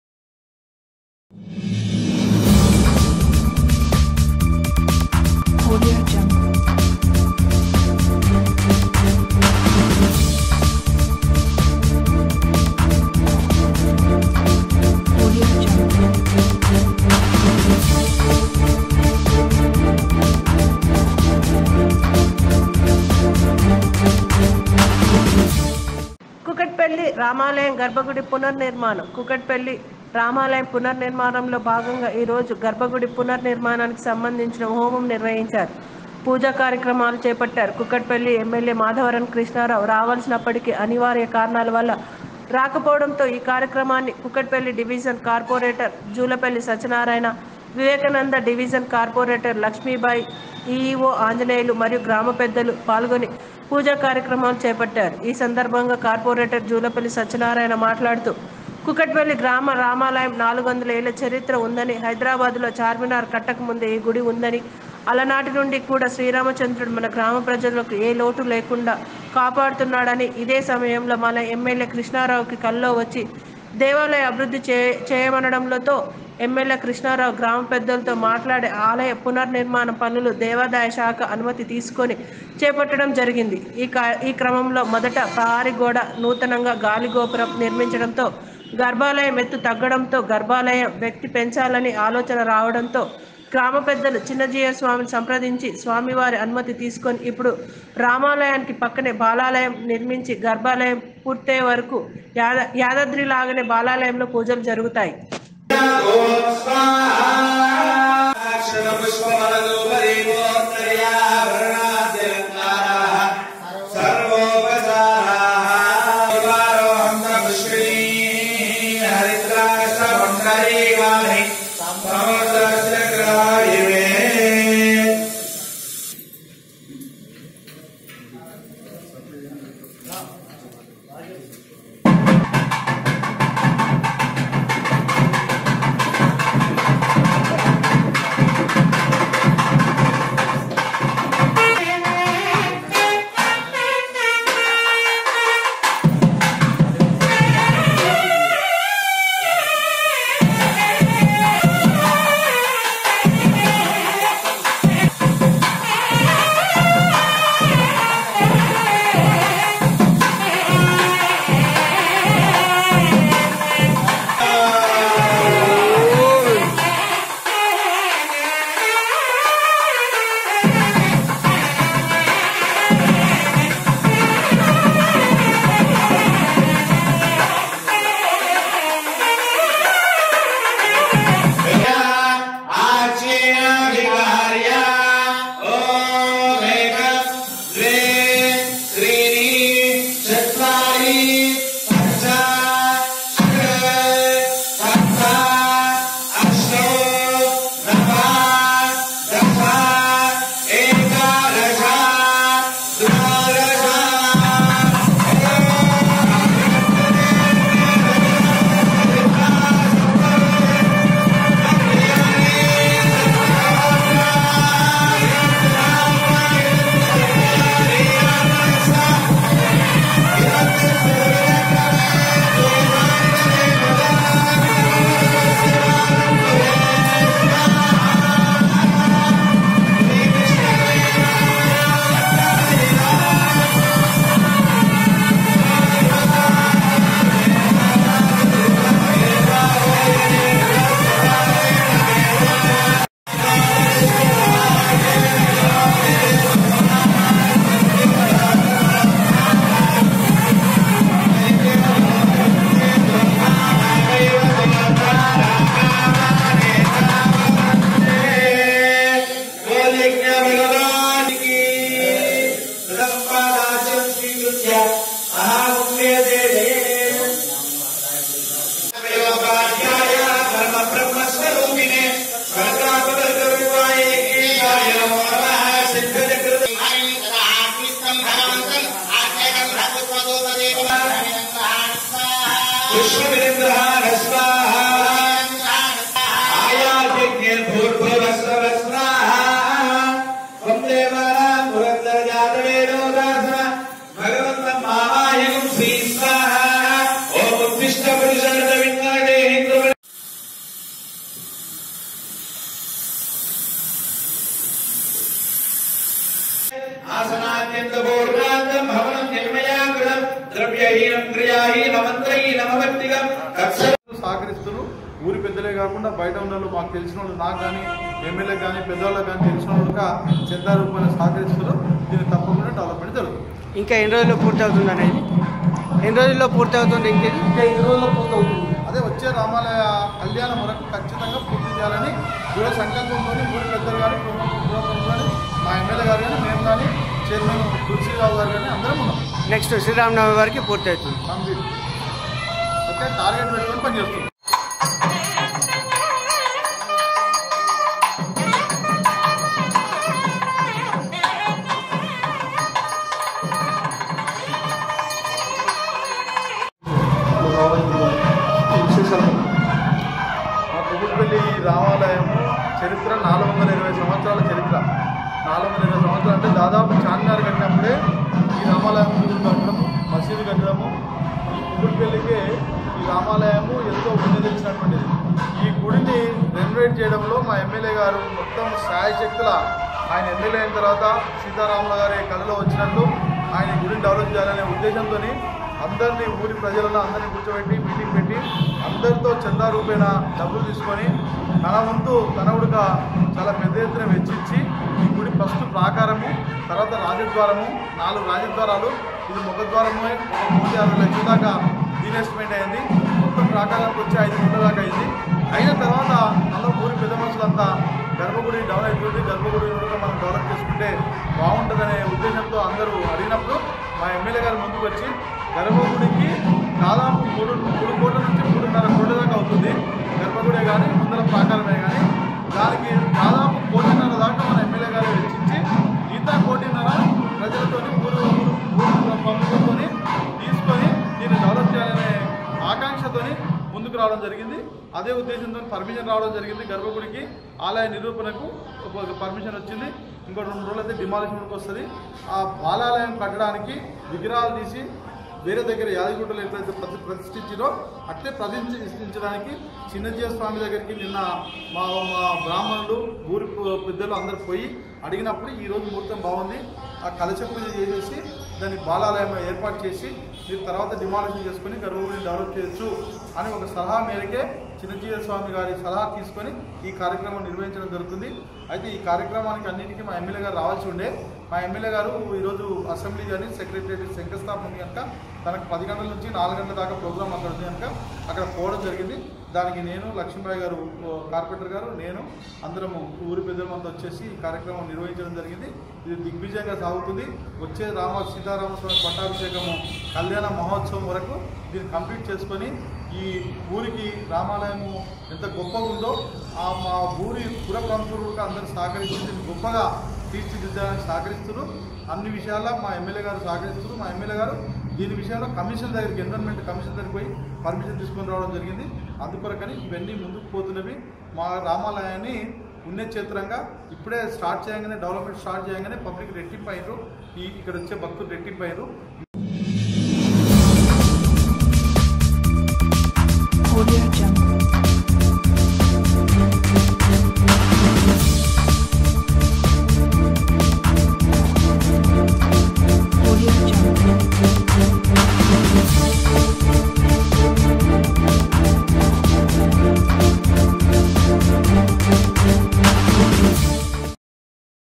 కూకట్ పల్లి రామాలయం గర్భగుడి పునర్నిర్మాణం....
జూన్27:-( సాక్షిత న్యూస్) కూకట్ పల్లి రామాలయం పునర్నిర్మాణం లో బాగంగా ఈ రోజు గర్భగుడి పునర్నిర్మాణానికి సంబంధించిన హోమం  నిర్వహించి పూజా కార్యక్రమాలు చేపట్టారు,ఈ కూకట్ పల్లి ఎమ్మెల్యే మాధవరం కృష్ణారావు కావాల్సిన అప్పటికీ అనివార్య కారణాలవల్ల రాకపోవడంతో ఈ కార్యక్రమాన్ని  కూకట్ పల్లి డివిజన్ కార్పొరేటర్ జూపల్లి సత్యనారాయణ,వివేకానందనగర్ డివిజన్ కార్పొరేటర్ లక్ష్మీ భాయ్ ,ఈఓ ఆంజనేయులు మరియు గ్రామ పెద్దలు పాల్గొని పూజ కార్యక్రమాలు చేపట్టారు,ఈ సందర్బంగా  కార్పొరేటర్ జూపల్లి సత్యనారాయణ మాట్లాడుతూ,కూకట్ పల్లి గ్రామ రామాలయనికి 400 ఏళ్ల చరిత్ర ఉందని హైదరాబాదులో చార్మినార్ కట్టకముందే ఈ గుడి ఉంది అని ఆనాటినుండి కూడా శ్రీరామచంద్రుడు గ్రామ ప్రజలకు ఏ లోటు లేకుండా కాపాడుతున్నాడని,ఇదే సమయం లో మా ఎమ్మెల్యే కృష్ణారావు కు కలలో వచ్చి దేవాలయ అభివృద్ధి చేయమనడం తో ఎమ్మెల్యే కృష్ణారావు గ్రామ పెద్దలతో మాట్లాడి ఆలయ పునర్నిర్మాణ పనులు దేవాదాయ శాఖ అనుమతి తీసుకుని చేపట్టడం జరిందని ,ఈ క్రమంలో మొదటగా ప్రహరీ గోడ నూతన గాలిగోపురం నిర్మించడంతో గర్భాలయం ఎత్తు తగ్గడం తో గర్భాలయం వ్యక్తి పెంచాలని ఆలోచన రావడంతో గ్రామ పెద్దలు చినజీయర్ స్వామిని సంప్రదించి స్వామి వారి అనుమతి తీసుకుని ఇప్పుడున్న రాములవారికి పక్కనే బాలాలయం నిర్మించి గర్భాలయం పూర్తయ్యే వరకు యాదాద్రి లాగానే బాలాలయం లో పూజలు జరుగుతాయి,అలాగే గర్భాలయనికి దాదాపుగా మూడున్నర కోట్ల రూపాయల ఖర్చు వస్తుందని ఎమ్మెల్యే ద్వారా కోటి రూపాయల పైగా వరకు మరియు మిగతావి చందాల రూపం లో భక్తులే ఇస్తున్నారు,కాబట్టి పూర్తి నిర్మానమ్ శ్రీ రామనవమి వరకు పూర్తి అవుతుందని ఆయన తెలియచేసారు. ఈ మాజీ కార్పొరేటర్ కార్యక్రమానికి మాధవరం జంగారావు, ఈవో ఆంజనేయులు, నాయీనేని తులసి రావు, గుడి మాజీ చైర్మన్ యాదగిరి రావు, ములుగు ఆనందరావు మరియు బాబురావు గుడి నిర్మాణదారులు విశ్వనాథ చారి గ్రామ పెద్దలు ,మహిళలు పాల్గొన్నారు